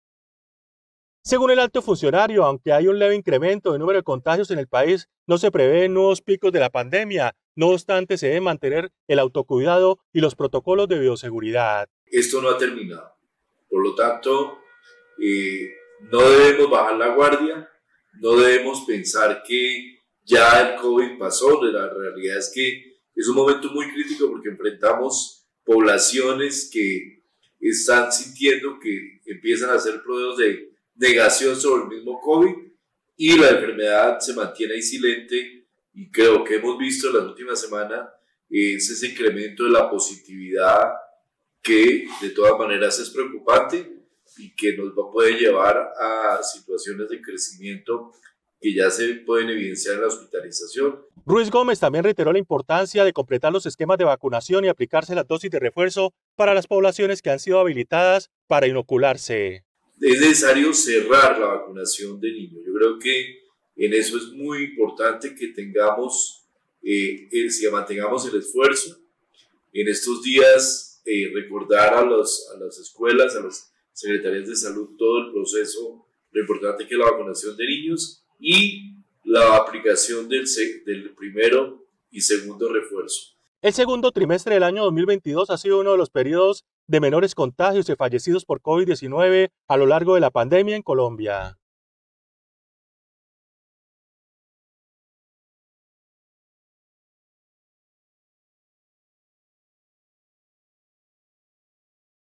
No se prevén nuevos picos de Covid-19: Ministro de Salud | Oro Noticias
Sin embargo, el funcionario pidió no bajar la guardia, ya que en las últimas semanas se ha presentado un aumento en la positividad de la enfermedad; así mismo, hizo un llamado a completar los esquemas de vacunación, especialmente en la población infantil.